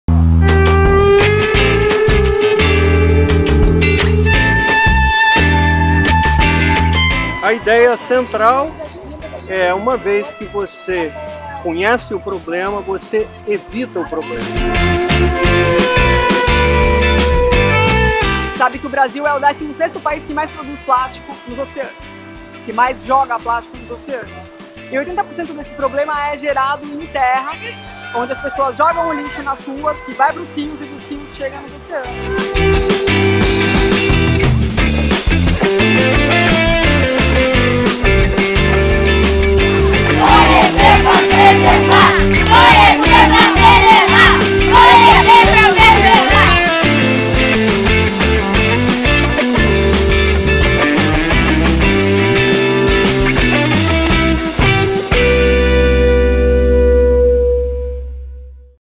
Clean Up Conhecer para Preservar Copacabana - WWF Brasil e Instituto Mar Adentro
No dia 25 de Março mais de 500 pessoas se reuniram na praia de Copacabana, no Rio de Janeiro, para um mutirão de limpeza da praia e conscientização das pessoas sobre o problema do lixo urbano e poluição dos oceanos por resíduos sólidos. A Cumulus TV esteve por lá com o Projeto Verde Mar produzindo conteúdo para a WWF Brasil e Instituto Mar Adentro. Veja um pouco do que rolou.